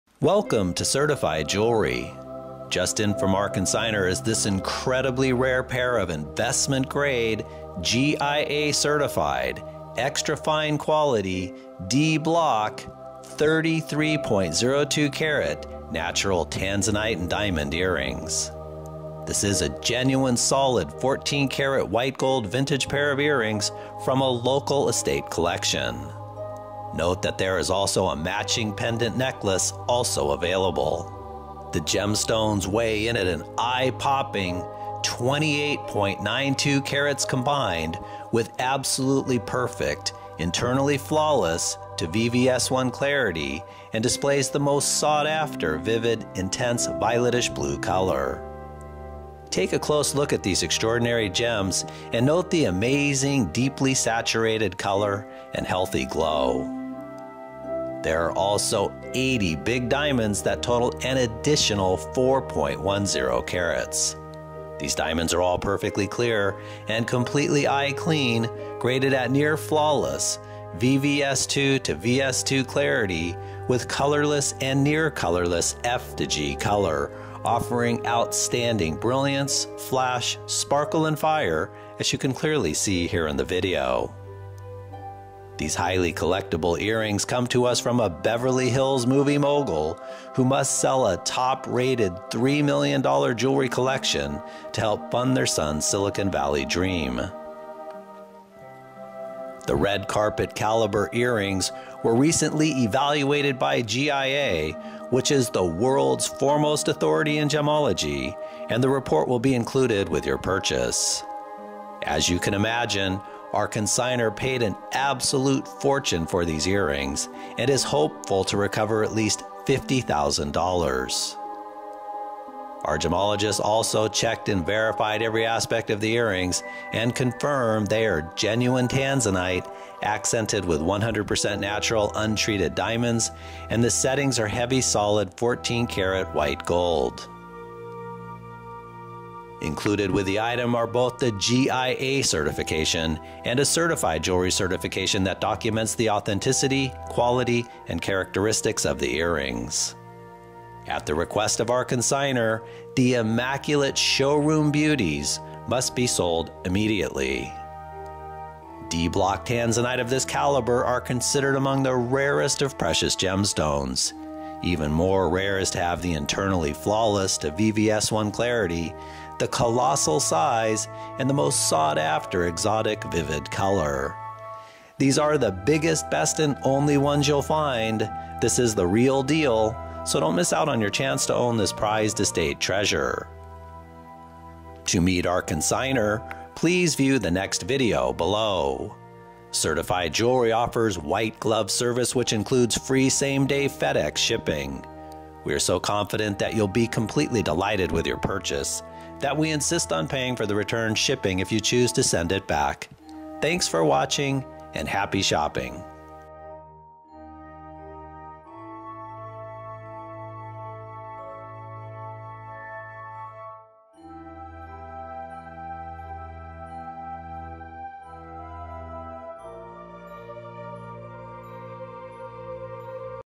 GIA Certified FLAWLESS Natural Tanzanite Diamond 14k White Gold Cocktail Stud Earrings - A131585
Top Gem AAA Quality GIA Certified 33.02 carat GIA Certified Tanzanite & Diamond Estate Earrings. These absolute TOP Quality Tanzanite gemstones boast the collector's grade vivid and intense deeply saturated Violetish Blue color, extra large 28.92 carat combined carat weight and to die for "Flawless to VVS1" clarity. Only the finest Tanzanite Specimens are a true deeply saturated violetish blue color and these beauties have it all and are not shy when it comes to sparkle and fire.

These rare gemstones are found in only one location on the planet making Tanzanite one of the most highly prized of all collectible gemstones. These world class gemstones are set in solid 14k white gold and feature an impressive 4.10 carats of top quality near flawless VVS-VS clarity "Colorless & Near Colorless" diamonds. These earrings are extraordinary in every way...the perfect addition to any high end fine jewelry collection. Not to be missed!!!